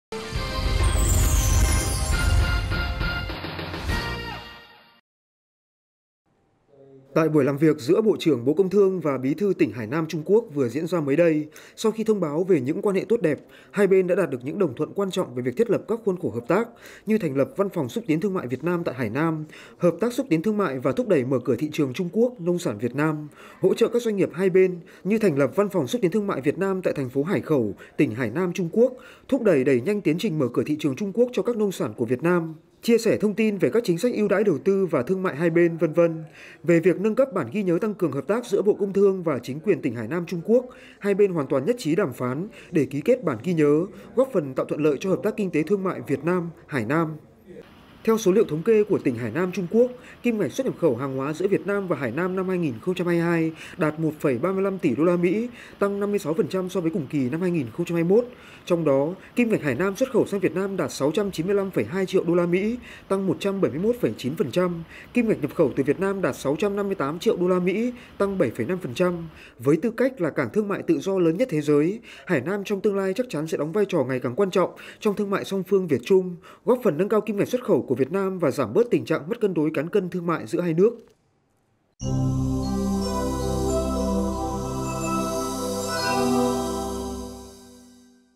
Thúc đẩy quan hệ thương mại Việt Nam – Hải Nam (Trung Quốc)
Tại buổi làm việc giữa Bộ trưởng Bộ Công Thương và  Bí thư Tỉnh Hải Nam – Trung Quốc vừa diễn ra mới đây, sau khi thông báo về những quan hệ tốt đẹp, hai bên đã đạt được những đồng thuận quan trọng về việc thiết lập các khuôn khổ hợp tác…